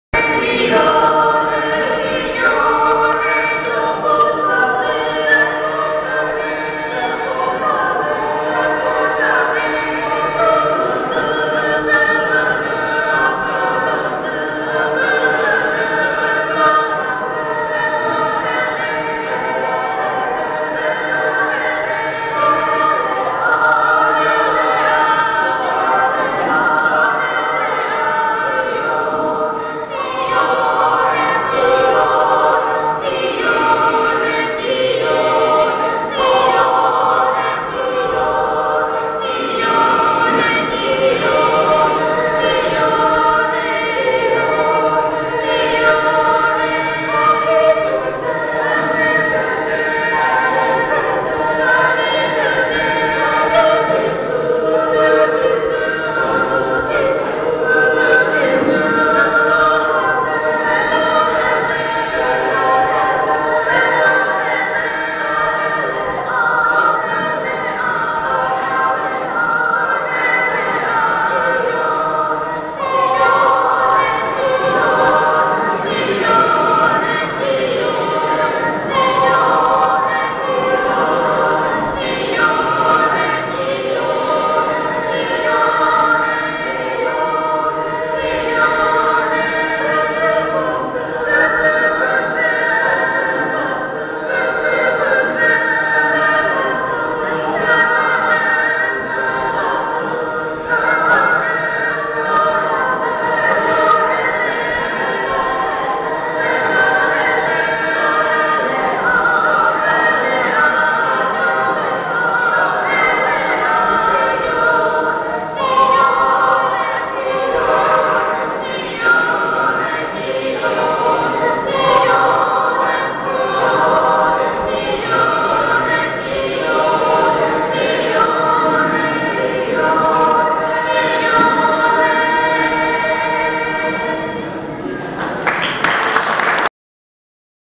Naga Folk Song
The Naga song was sung during "Asia Communication Week - 2010"